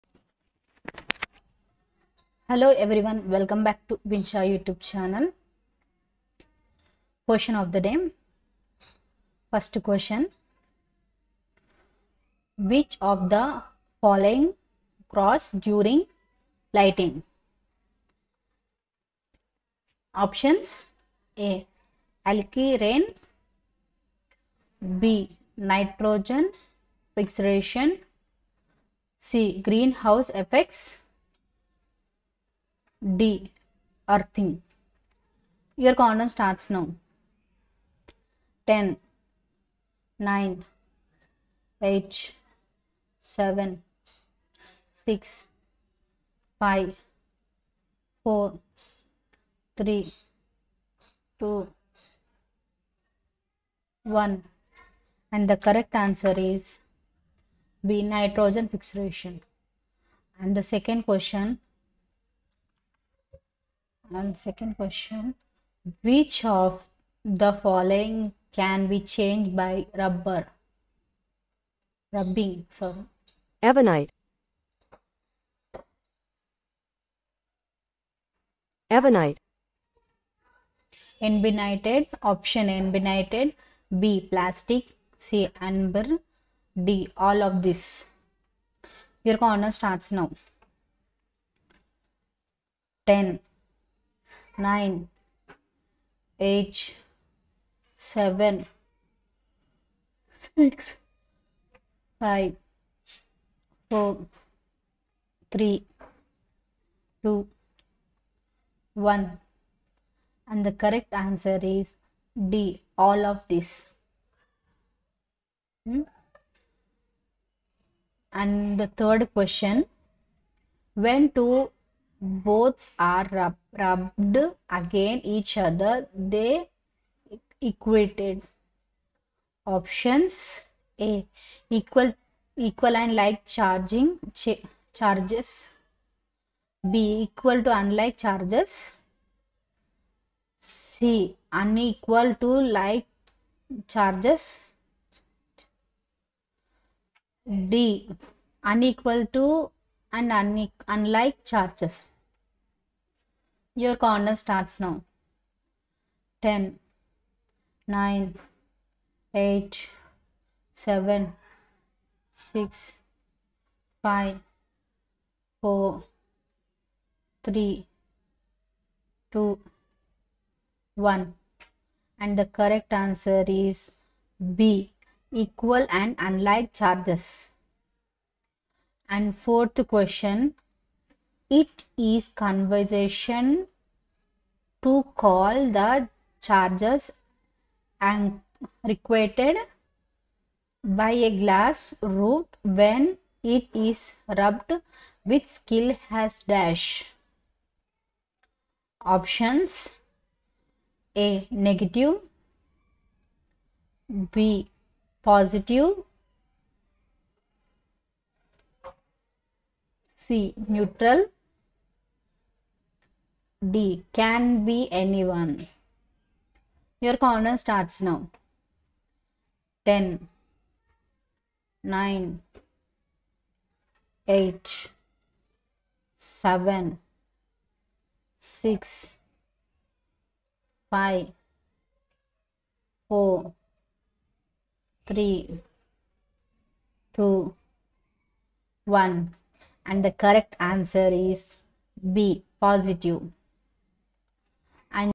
Which of the following occurs during lightning?
Question 13.
(a) Acid rain
(b) Nitrogen fixation
(d) Earthing

Answer
Answer: (b) Nitrogen fixation

Question 14.
Which of the following can be charged by rubbing?
(a) Ebonite
(b) Plastic

Answer
Answer: (d) All of these

Question 15.
When two bodies are rubbed against each other, they acquire
(a) equal and like charges
(b) equal and unlike charges
(d) unequal and unlike charges

Answer
Answer: (b) equal and unlike charges

Question 16.
It is a convention to call the charge acquired by a glass rod when it is rubbed with silk as __________
(a) Negative
(b) Positive
(d) Can be any one

Answer
Answer: (b) Positive